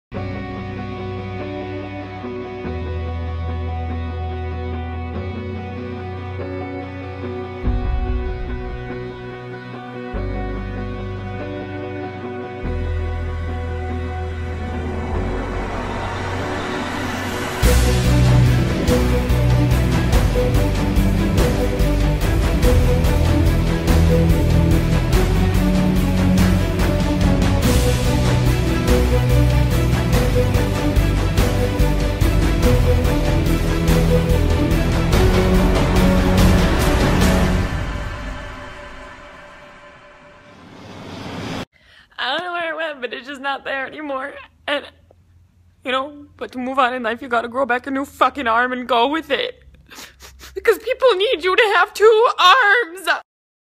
The Mark Of Athena (Unofficial) Trailer!
Hope you're all excited!! (This trailer was a joke made by Haley & Ashley but thanks for your support anyway!)